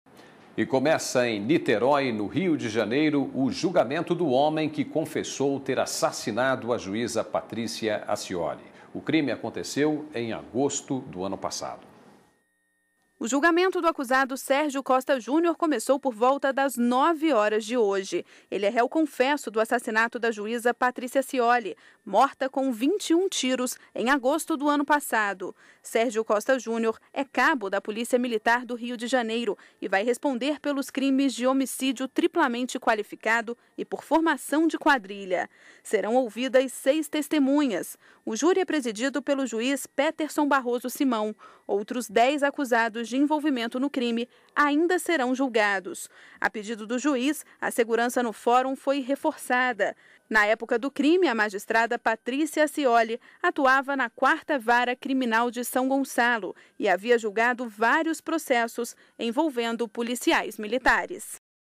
Julgamento do assassinato de Patrícia Acioli (04/12/12)
Começa em Niterói, no Rio de Janeiro, o julgamento do homem que confessou ter assassinado a juíza Patrícia Acioli. O crime aconteceu em agosto do ano passado.